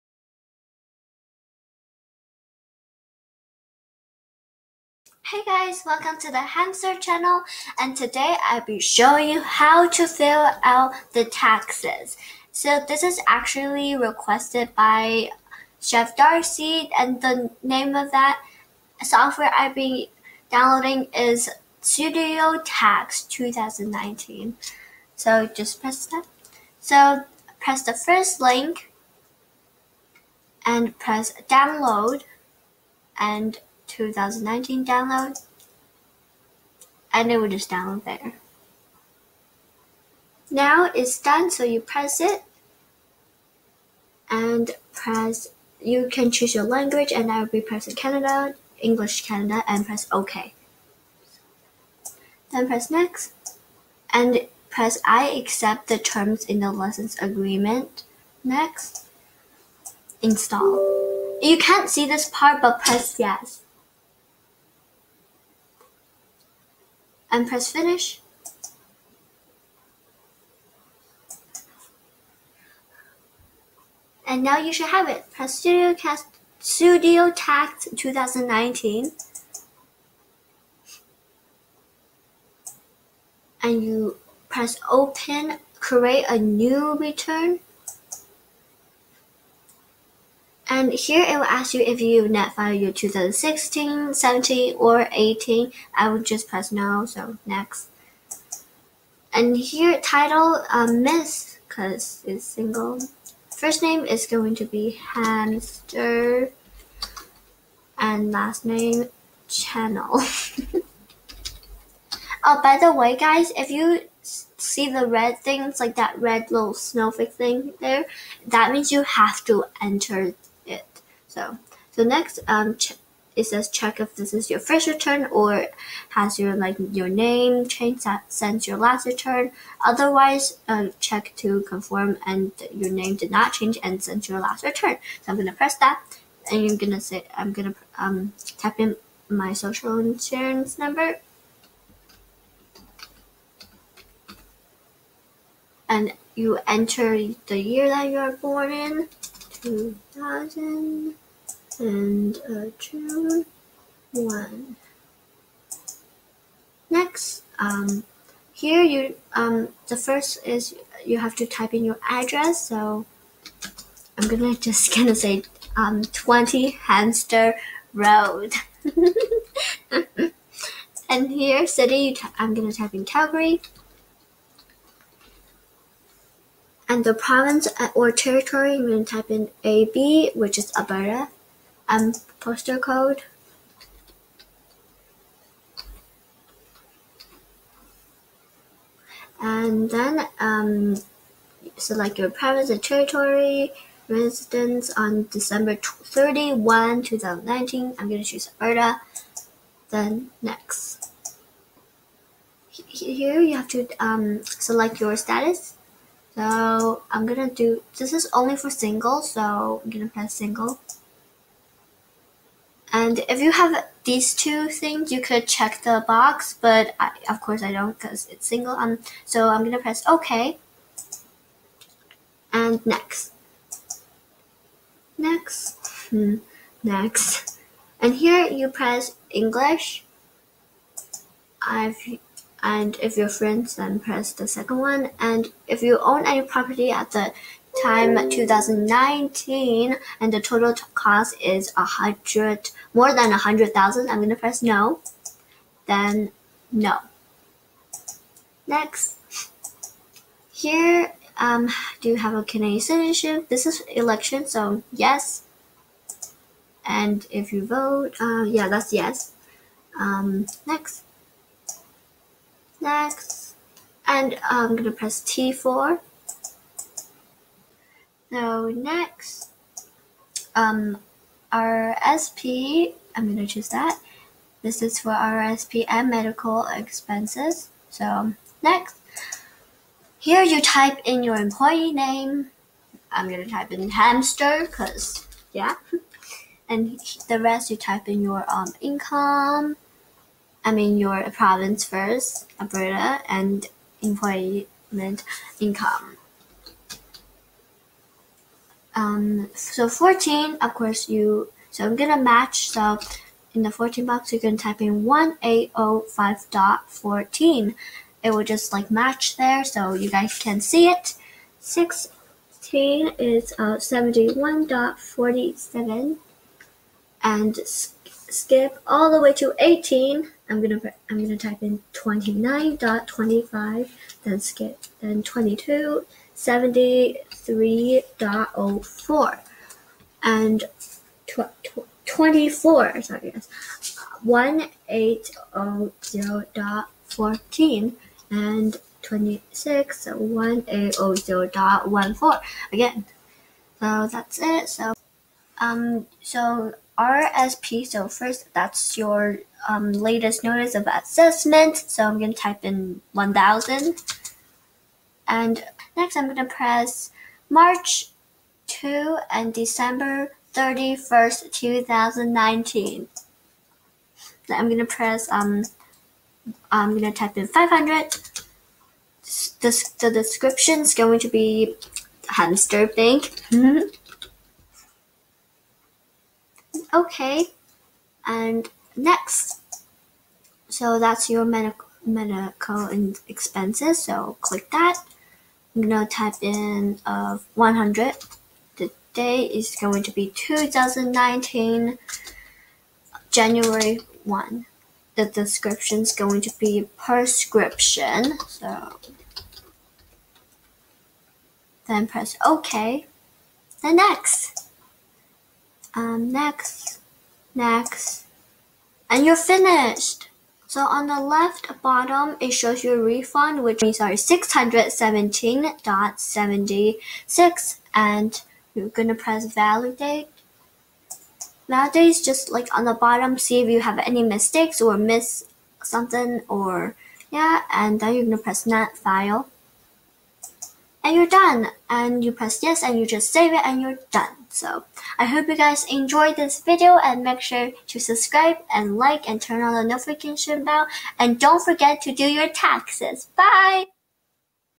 StudioTax: How to File Canada income tax for free
In this tutorial, I will show you how to use a free tax software as Studio Tax to file your income tax step by step. The tutorial are including a T4 Slip & RRSP contribution and Medical Expense.

Don't forget to file your tax before deadline.

If you have any computer topic that you would like to learn, please feel free to let me know